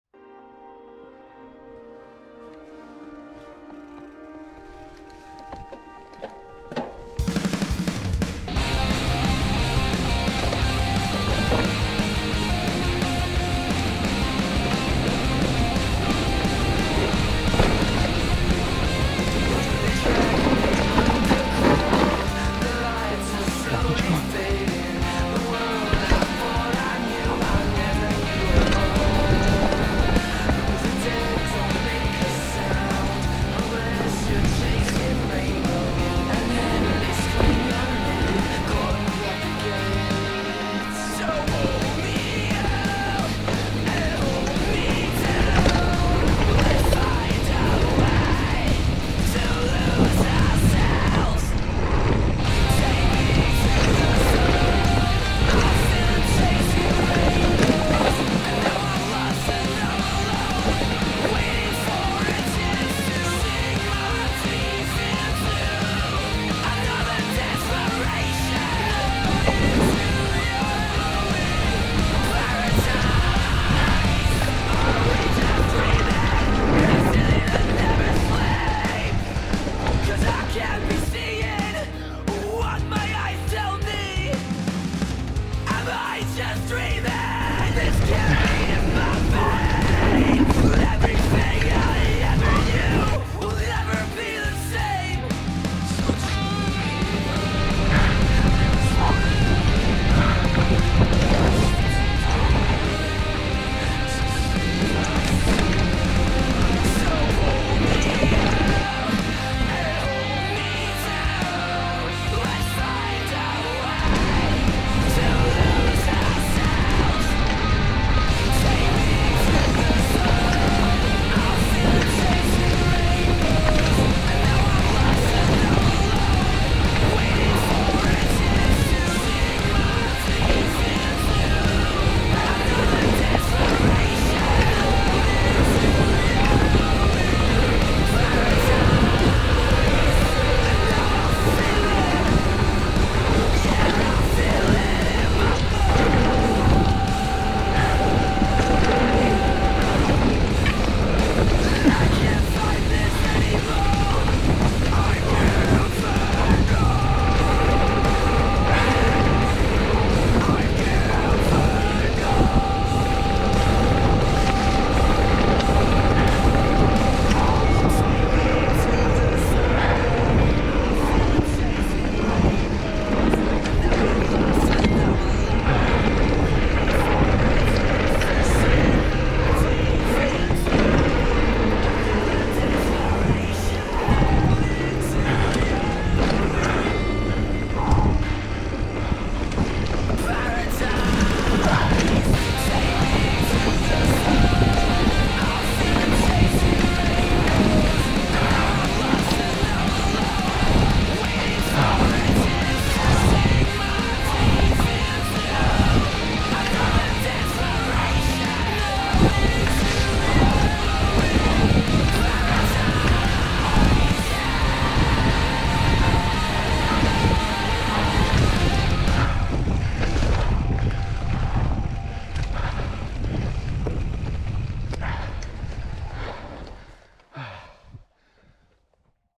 Chochola trail
Oblíbený trailík kousek od Brna. Bohužel jsem si kousek videa smazal, tak jsem to musel lepit z další jízdy.